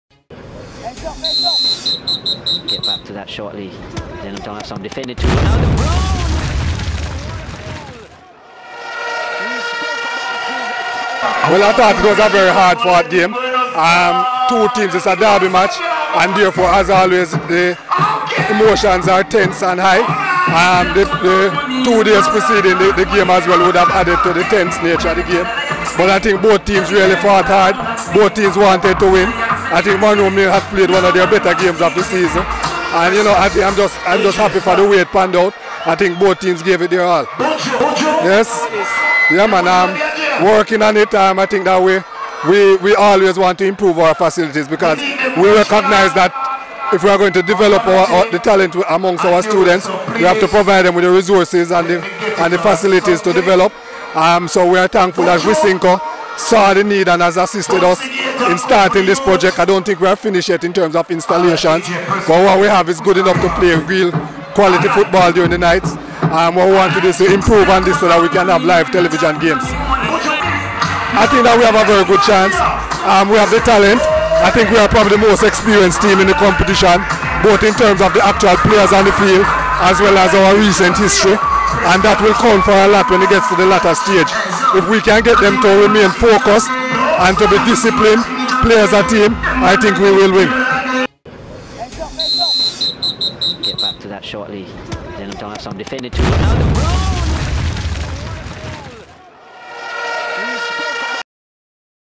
Post Game interview with STETHS Principal Kieth Wellington
Kieth Wellington Principal of St. Elizabeth Technical High is pleased with his schools' performance in their derby match against Munro College on Saturday the 30th of September. The Game ended 2-1 in favour of STETHS